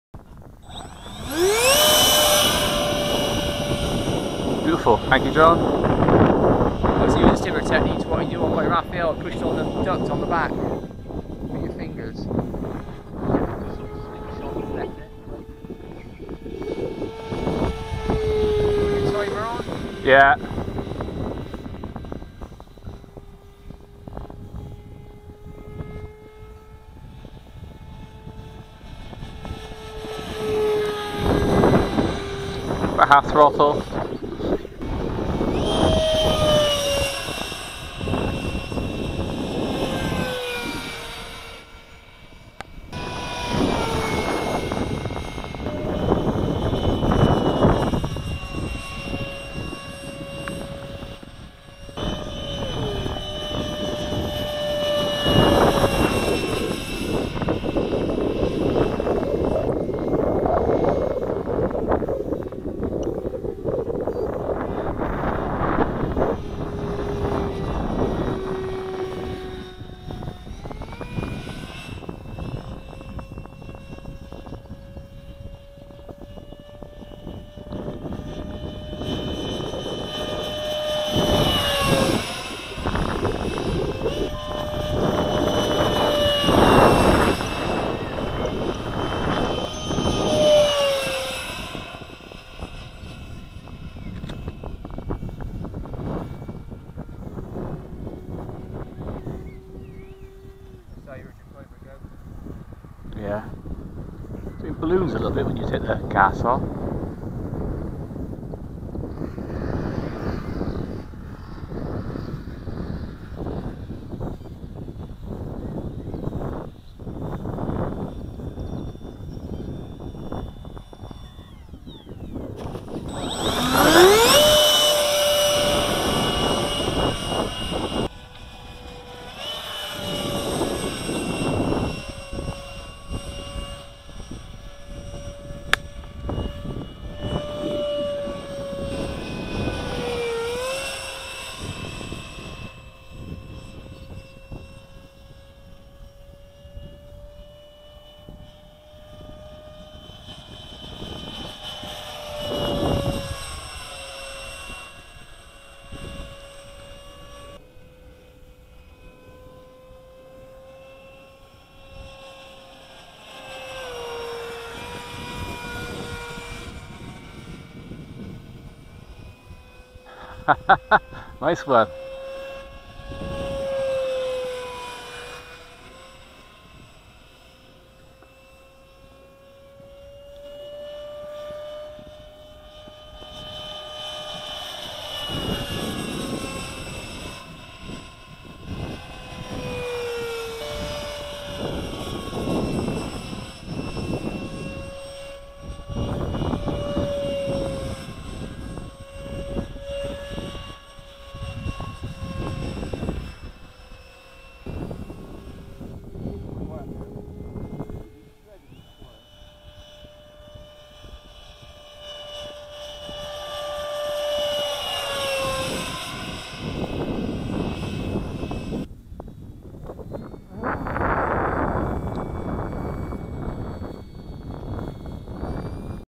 Durafly D.H.100 Vampire Mk6 EDF radio control Jet
A couple of evening flights with my stock Vampire. Grass too long for the small wheels so my brother obliged with a hand launch. Very easy model to fly as it has no bad habits and just floats in for a smooth touch down.
Video shot with my hat mounted Mobius on fw v.1.2, 60 fps,720p